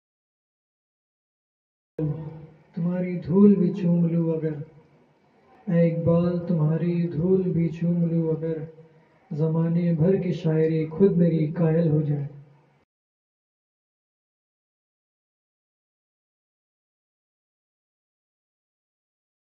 Shayari | ae iqbal..
A tribute to Sir # allama iqbal .
A shayari / poetry showing love & respect for sir iqbal .